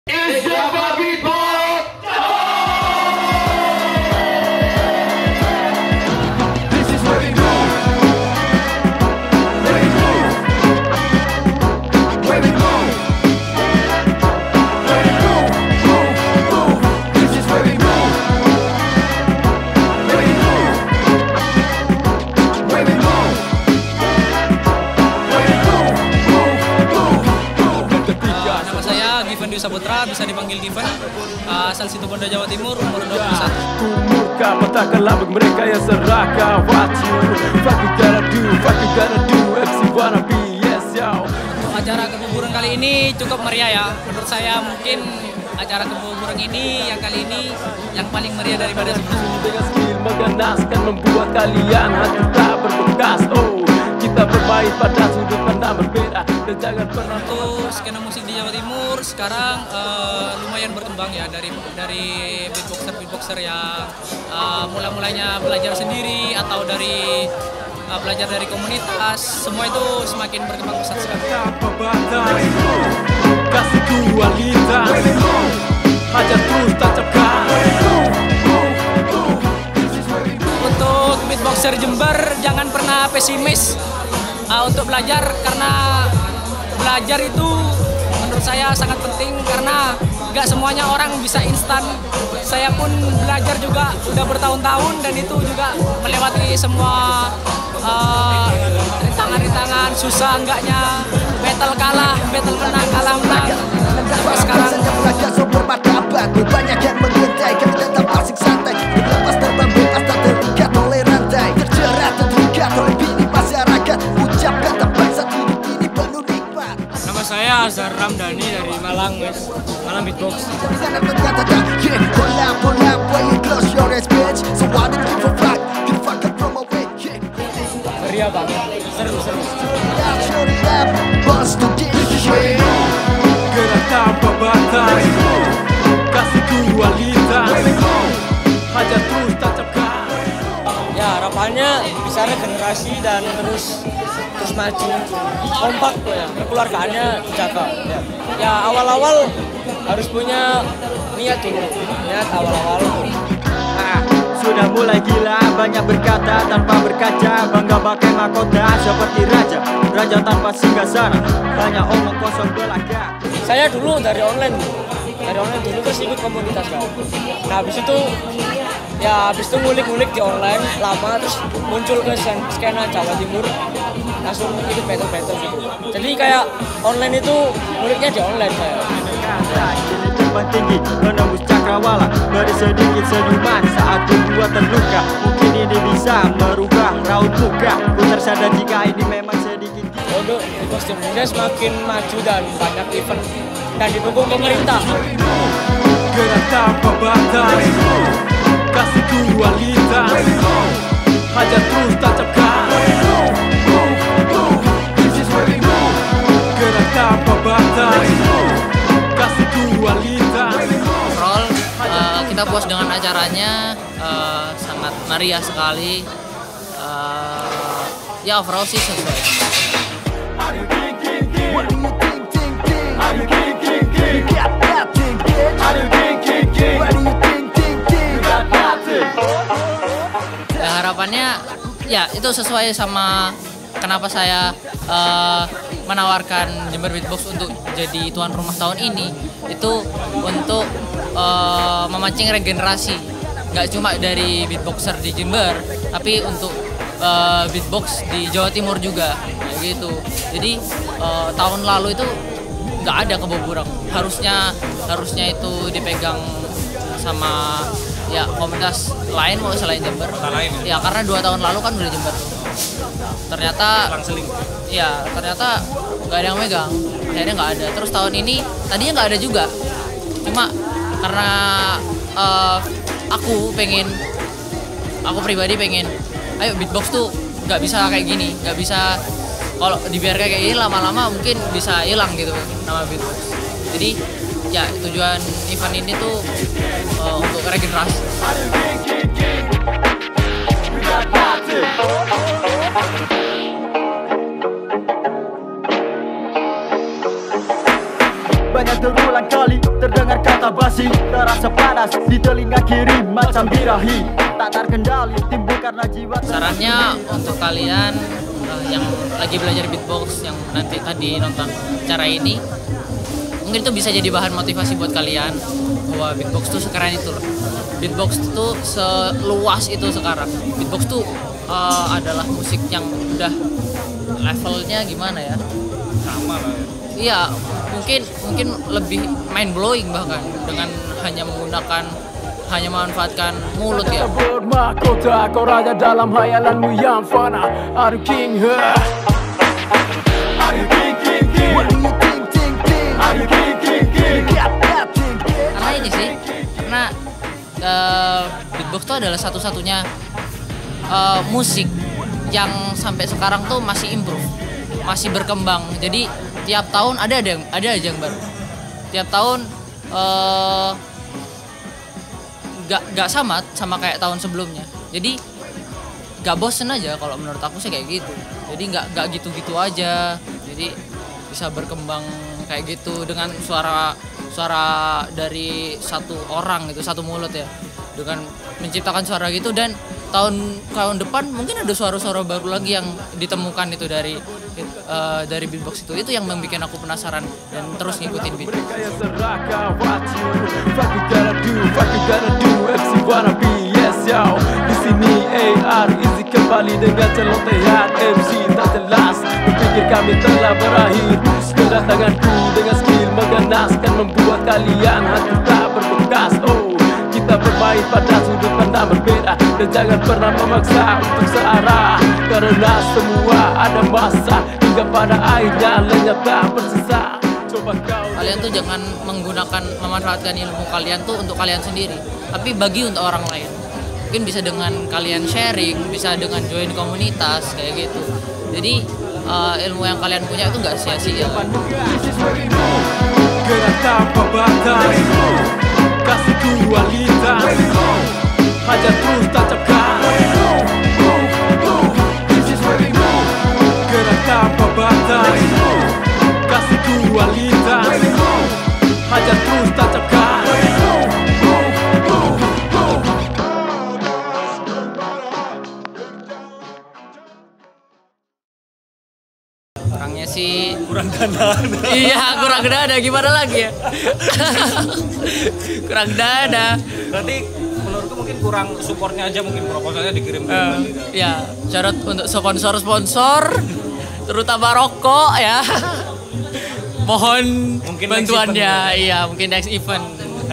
KEBO BURENG VOL.6 | Keluarga Beatbox Buko Bareng by Jember Beatbox
Kali Ini Kita Di Ajak Temen-Temen Jember Beatbox & East Java Beatbox Untuk Datang Di Event Tahunan (Kebo Bureng Vol.6) Mereka,
Dan Kita Juga Berkesempatan Ngobrol Dengan Judges (Given), 1st Winner Battle (Azzar) Dan Juga Promotor Event (Rama) Di Event Tersebut.